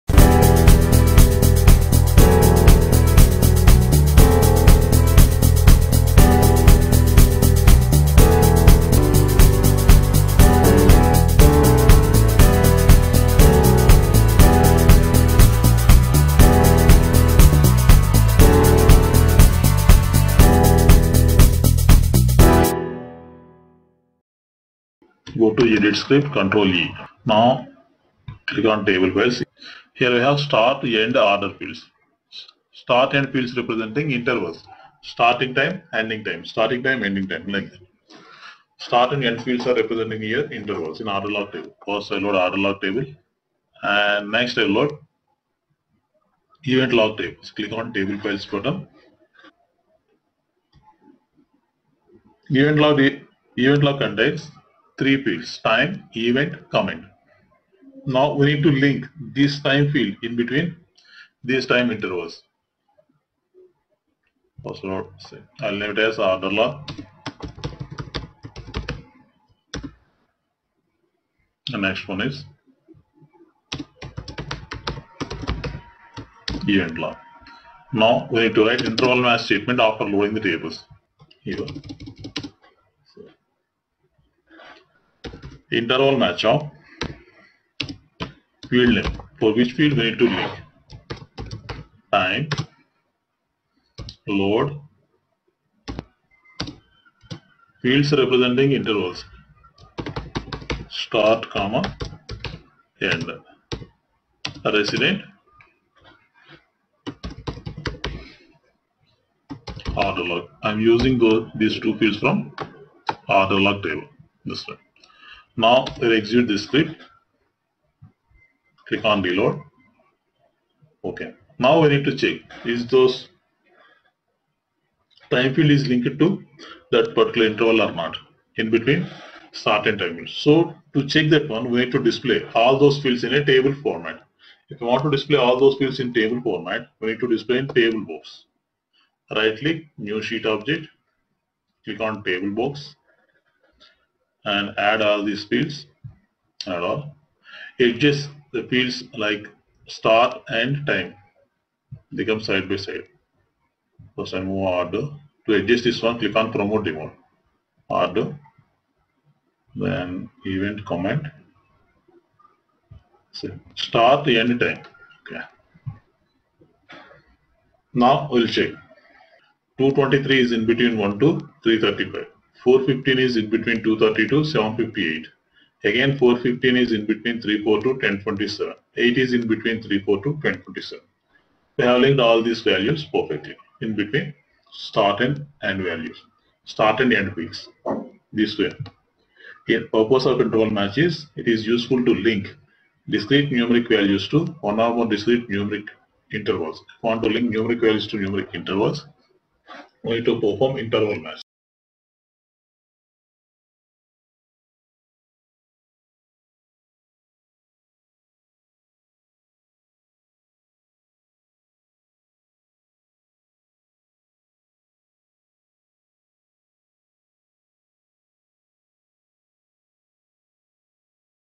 Interval Match - QlikView Tutorial - Session 11
In this session you will learn about Interval Match

Intervalmatch
- Useful to link discrete numeric values to one or more discrete numeric intervals

Conditions
- Tables must be loaded before writing Intervalmatch Statement
- One Table must contain 'Start' and 'End' Type of Fields
- Intervals must be closed

1-10  9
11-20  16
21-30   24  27

24
16
9
27

OrderLog - Order,Start,End
EventLog - Event, Time,Comment

Intervalmatch(Time) Load Start,End Resident OrderLog;